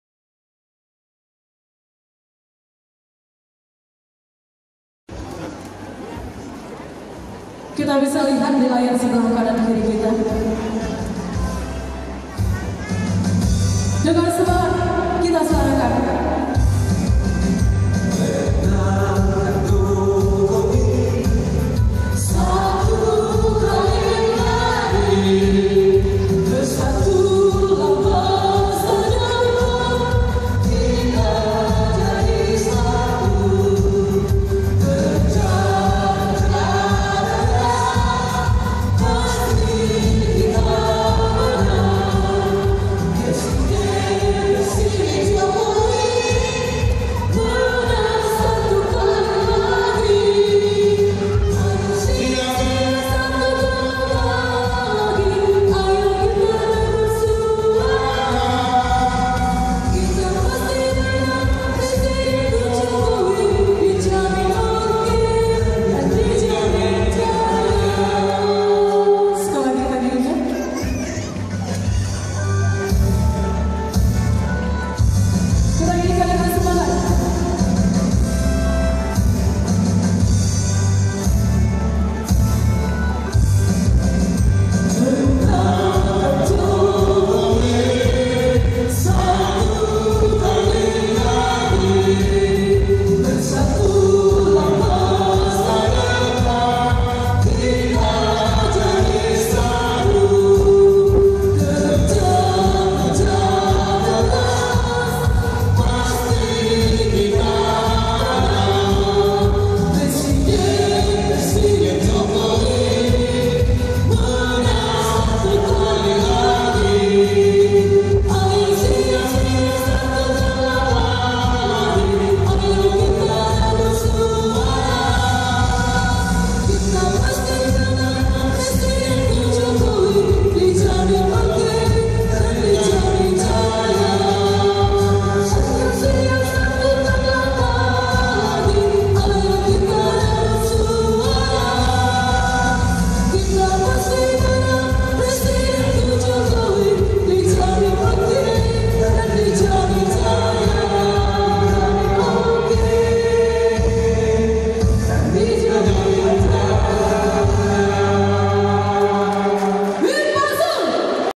Kereeen banget lagu Deklarasi dukungan Himpasu untuk Jokowi - Ma'ruf Amin
Deklarasi dukungan Himpunan Masyarakat Prasejahtera Sumatera Utara kepada Jokowi - Ma'ruf Amin,  Pardede Hall, Medan 24 Februari 2019.